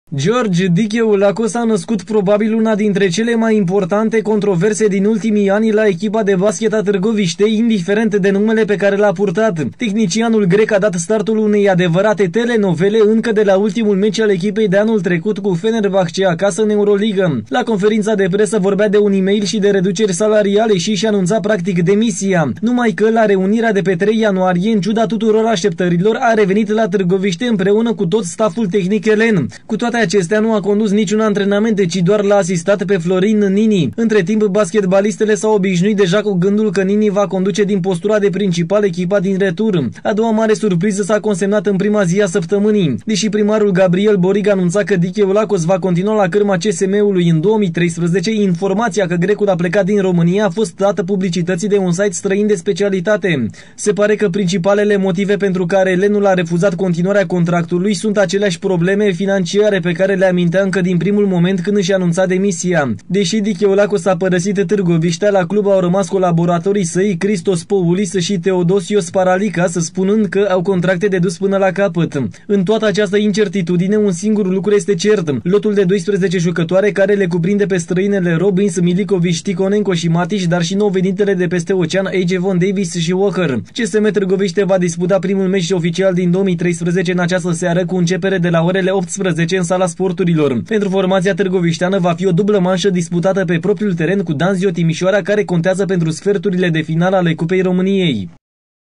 Telenovela Dikeoulakos (Columna Tv)
Cazul lui George Dikeoulakos devine din ce in ce mai interesant. Incertitudinea creata la echipa imediat de dupa meciul cu Fenerbahce din Euroliga se prelungeste chiar si in primele zile ale anului 2013, cand Targovistea are o "dubla" decisiva cu Timisoara pentru semifinalele Cupei Romaniei. Desi a fost prezent la reunire, conform unor surse, antrenorul grec nu va mai ramane pe banca Municipalului in retur. George Dikeoulakos a nascut probabil una dintre cele mai importante "controverse" din ultimii ani la echipa de baschet a Targovistei, indiferent de numele pe care l-a purtat. Tehnicianul grec a dat startul unei adevarate "telenovele" inca de la ultimul meci al echipei de anul trecut, cu Fenerbahce acasa, in Euroliga.
   La conferinta de presa vorbea de un e-mail si de reduceri salariale si isi anunta practic demisia. Numai ca, la reunire, in ciuda tuturor asteptarilor, a revenit la Targoviste, impreuna cu tot staff-ul tehnic elen. Cu toate acestea, nu a condus niciun antrenament, ci doar l-a asistat pe Florin Nini. Intre timp, baschetbalistele s-au obisnuit deja cu gandul ca Nini va conduce din postura de "principal" echipa din retur.
   A doua mare surpriza s-a consemnat in prima zi a saptamanii. Desi primarul Gabriel Boriga anunta ca Dikeoulakos va continua la carma CSM-ului in 2013, informatia ca grecul a plecat din Romania a fost data publicitatii de un site strain de specialitate. Se pare ca principalul motiv pentru care elenul a refuzat continuarea contractului sunt aceleasi "probleme financiare" pe care le amintea inca din primul moment cand isi anunta demisia. Desi Dikeoulakos a parasit Targovistea, la club au ramas colaboratorii sai Christos Poulis si Theodosios Paralikas, spunand ca "au contracte de dus pana la capat".
   In toata aceasta incertitudine, un singur lucru este cert: lotul de 12 jucatoare, care le cuprinde pe strainele Robbins, Miljkovici, Tikhonenko si Matici, dar si nou venitele de peste "Ocean", Ajavon, Davis si Walker. CSM Targoviste va disputa primul meci oficial din 2013 in aceasta seara, cu incepere de la orele 18:00, in Sala "Sporturilor". Pentru formatia targovisteana va fi o "dubla" mansa disputata pe propriul teren, cu Danzio Timisoara, care conteaza pentru sferturile de finala ale Cupei Romaniei.